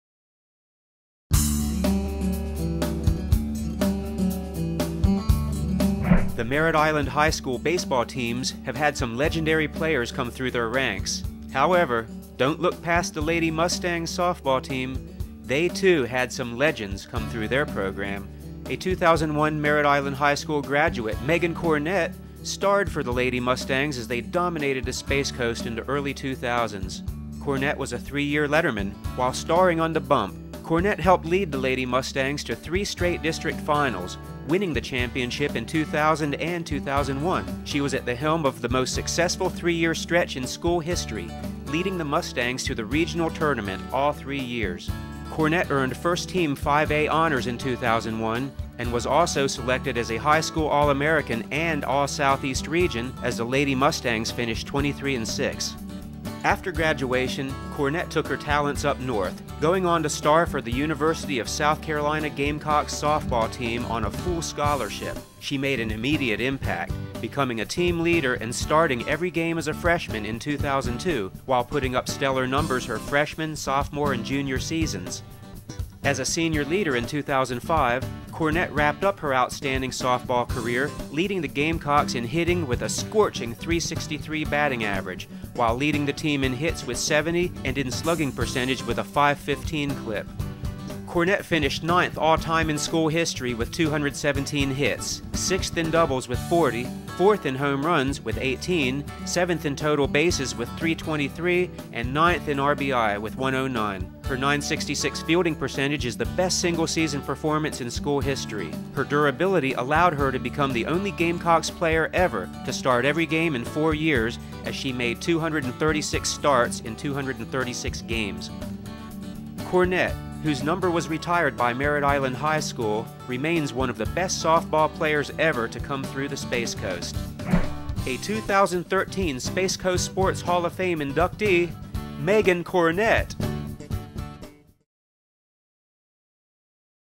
Space Coast Sports Hall of Fame 2013 - Meghan Cornett
Read about Meghan Cornett's incredible softball career at Merritt Island High & the University of South Carolina